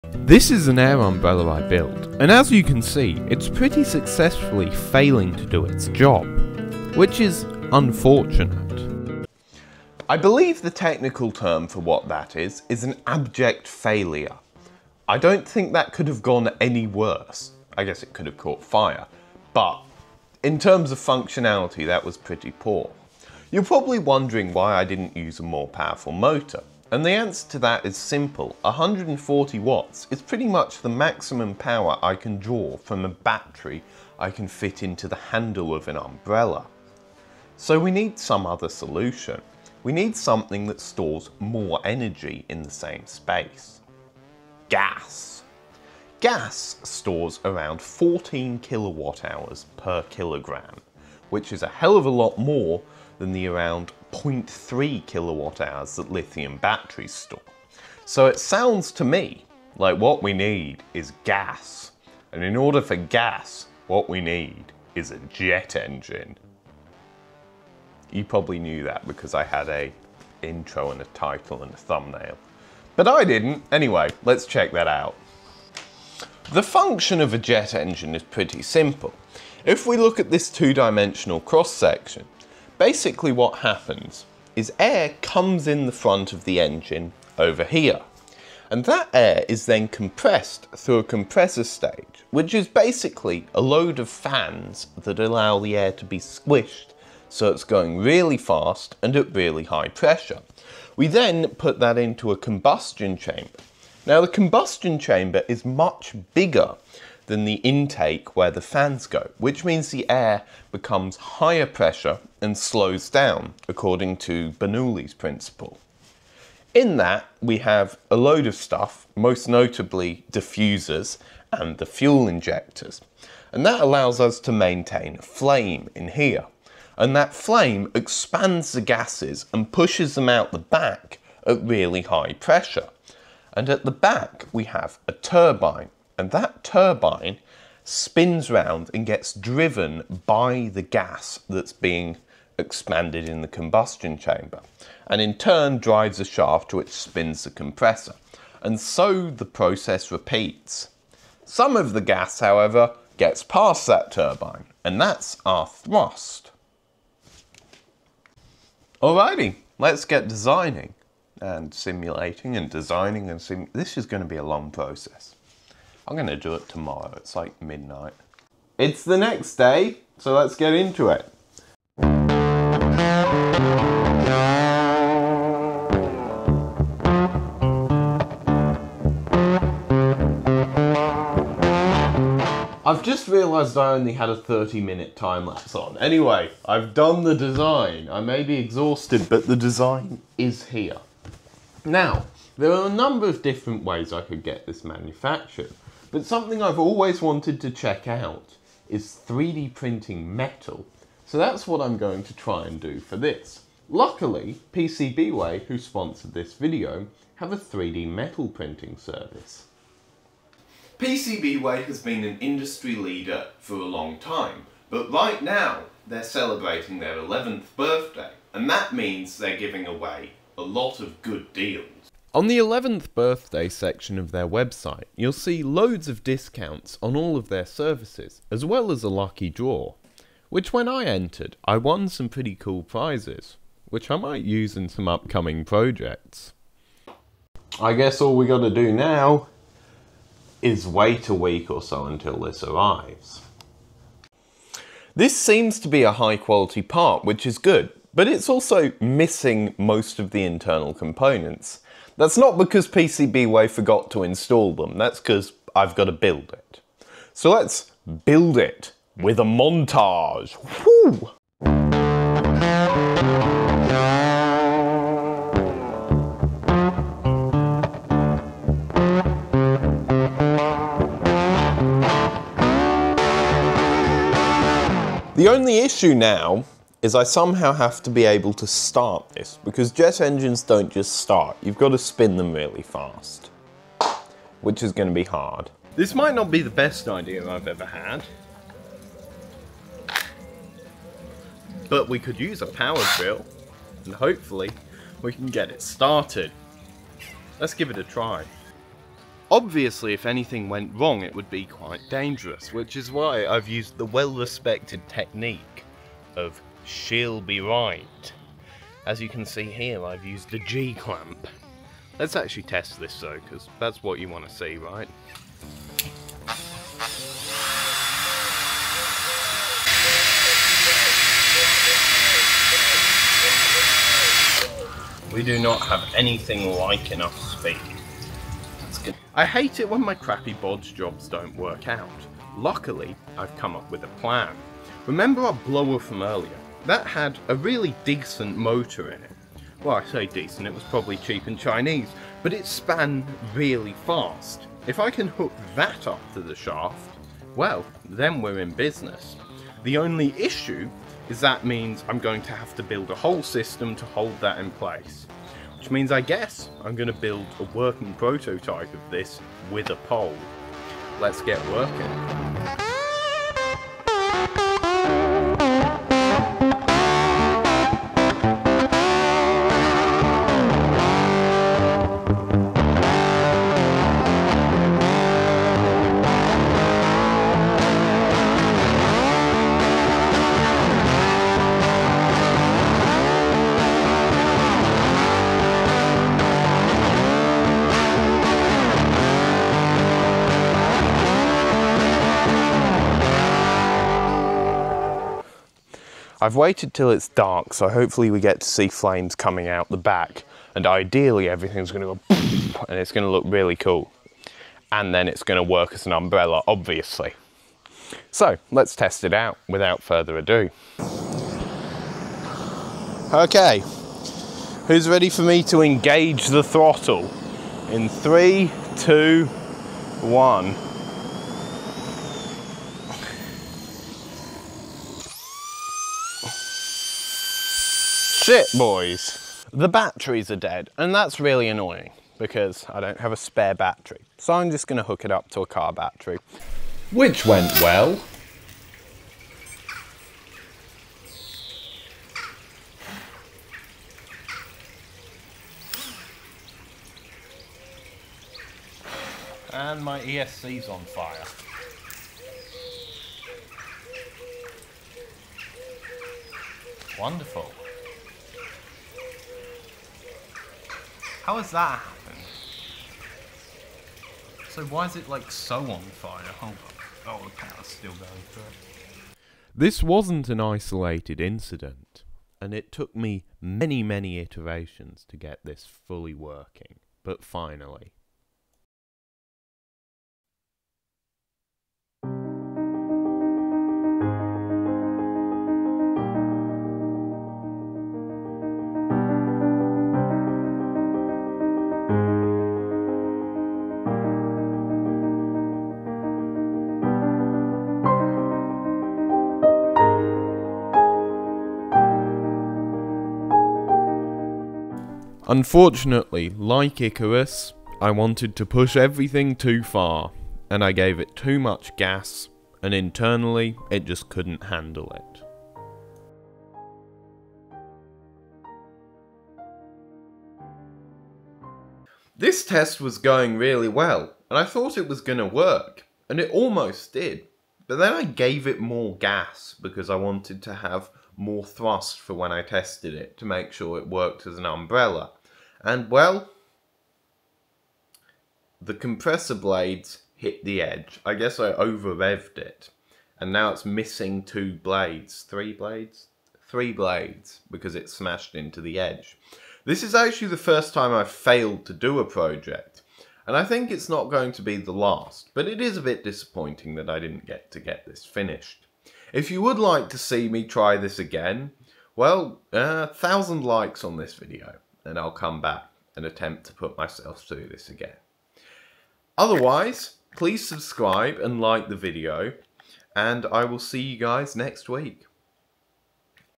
JET ENGINE POWERED AIR UMBRELLA (abr.)
In this video, I build an air umbrella using a jet engine. Unfortunately, this isn't as easy as just putting a jet on the pole. I had to build the world's smallest jet engine(as far as I know).
Also, if anyone with a jet engine wants to try this(I'm looking at you @hacksmith ), please do. I really want to see this idea come to fruition.

In a new innovation, I need to add a better starting mechanism.